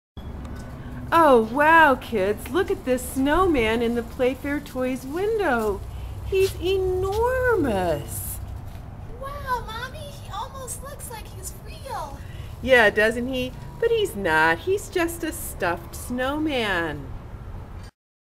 Snowflake is admired
Snowflake rests in the PlayFair Toys window, looking out at the world.